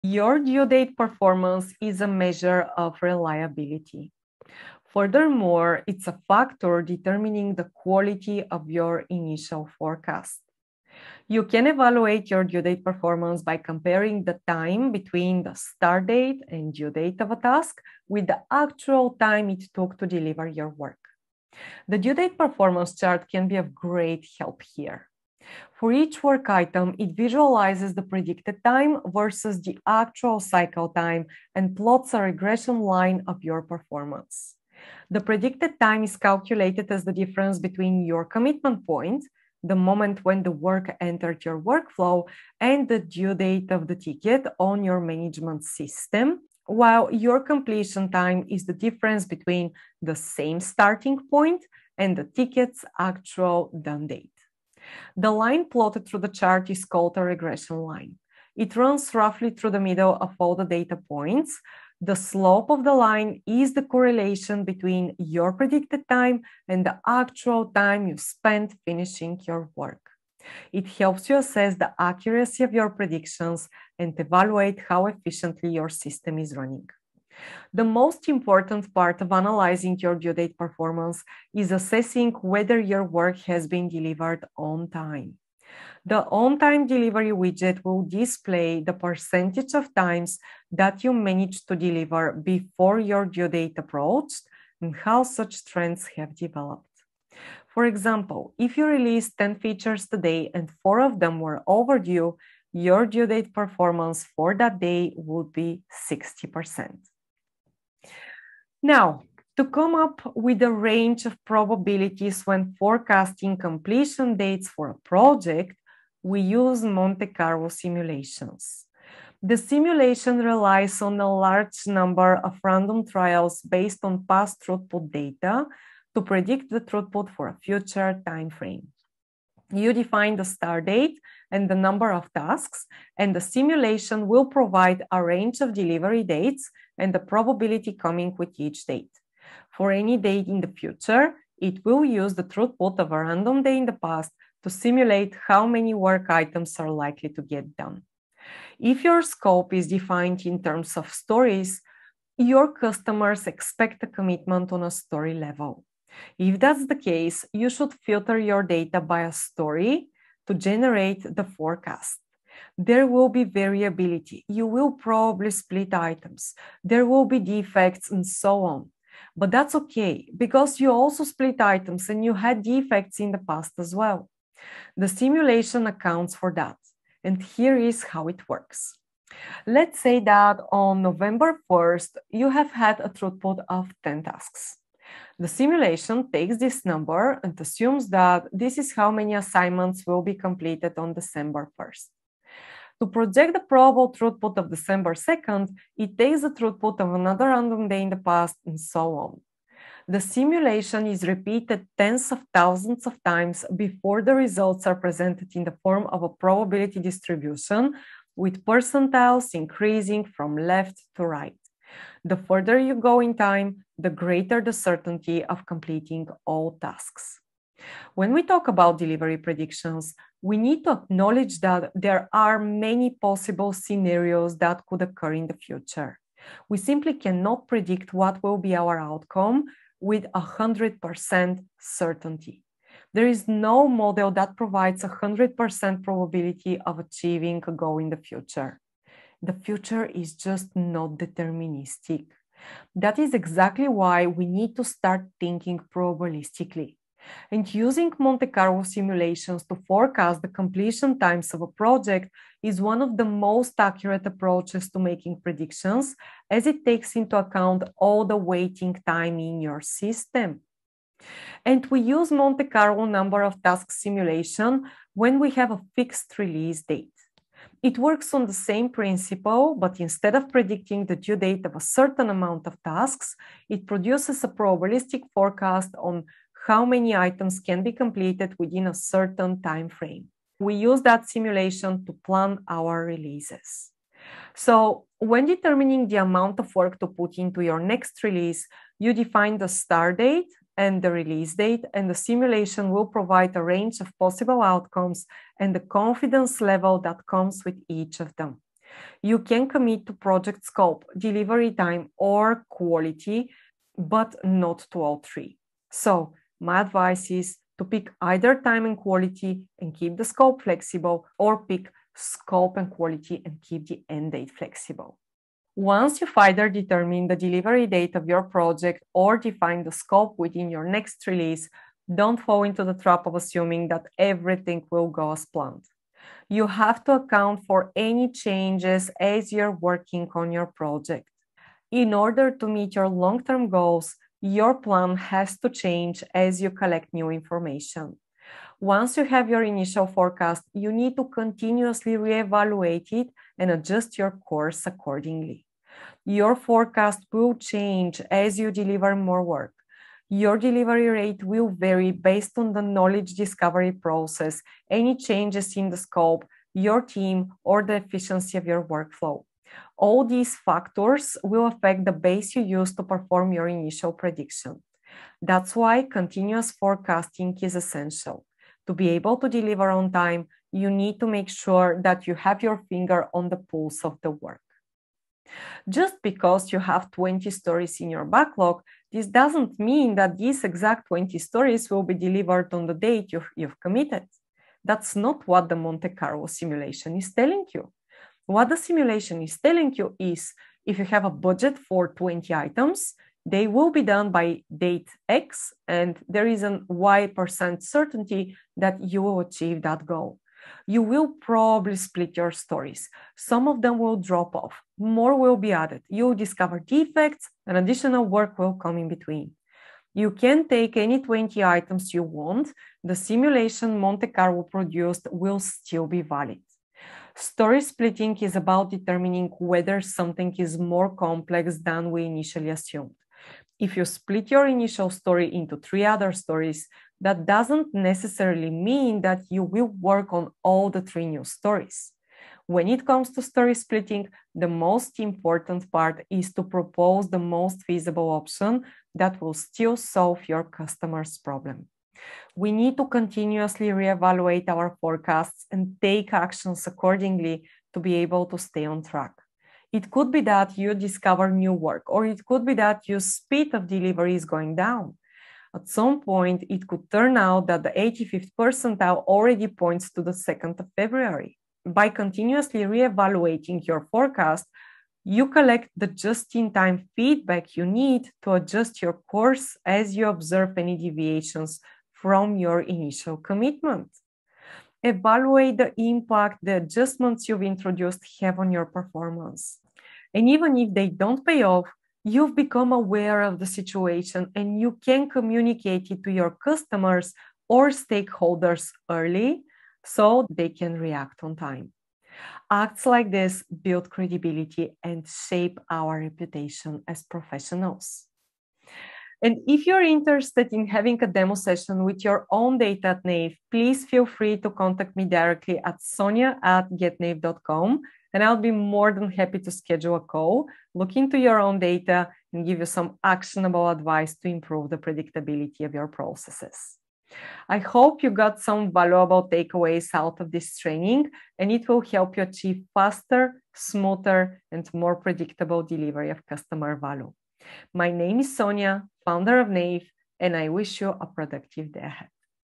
How to Use the Due Date Performance Chart and Monte Carlo Simulations by Nave
Let’s explore the concept of due-date performance and the most effective approach to making reliable data-driven delivery predictions: Monte Carlo simulations!